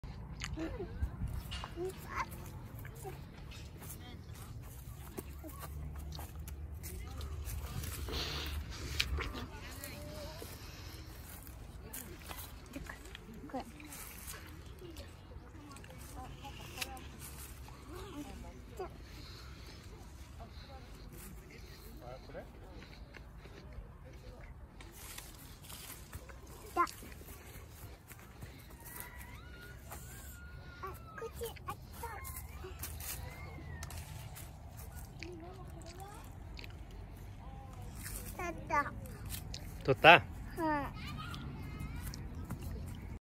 ApplePicking
Apple Picking at Numata City
Gunma
Someya Apple Park
Numata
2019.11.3
Sunday